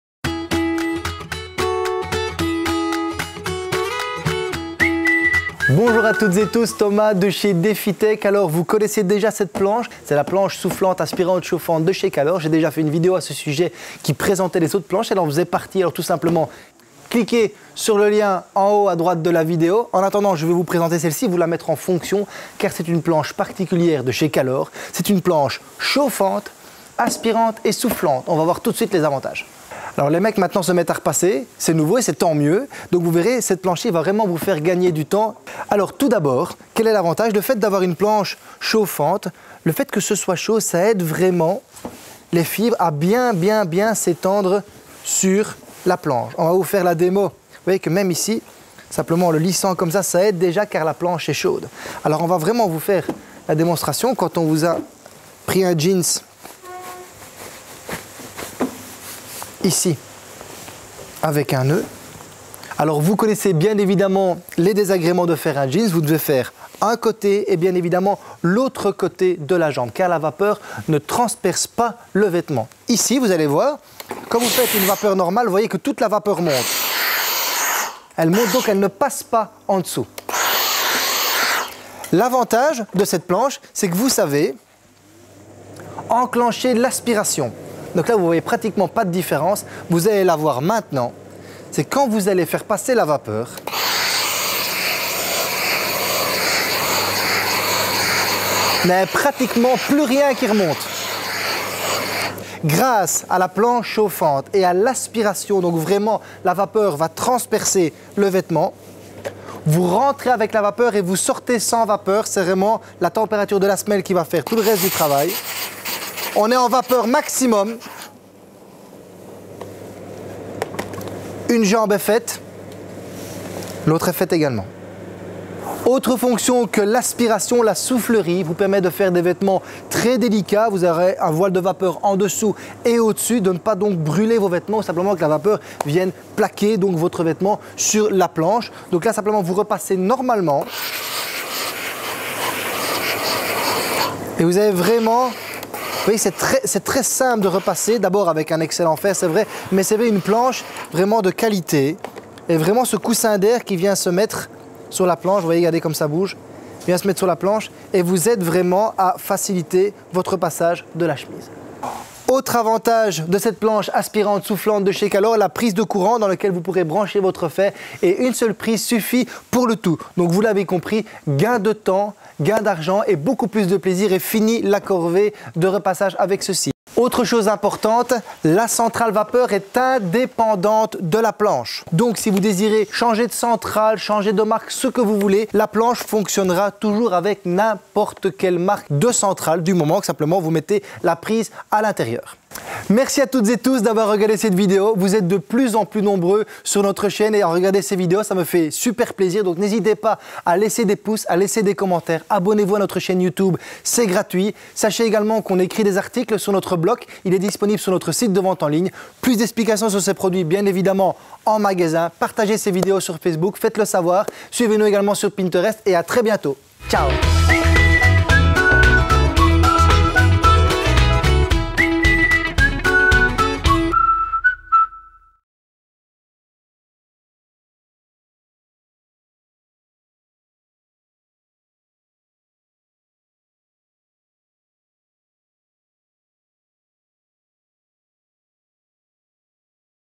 Quelle facilité ! Planche à repasser aspirante et soufflante de Calor - unboxing et démo Defitec
Les femmes qui repassent seules c’est un peu dépassé comme cliché, non ?
Et oui messieurs, un homme qui peut repasser une chemise c'est la classe ! Vous débutez ? Pas de panique, avec cette planche, vous donnerez l'impression d'avoir fait ça toute votre vie...

N'hésitez pas à commenter, me contacter ou passer me voir !

A bientôt,
T.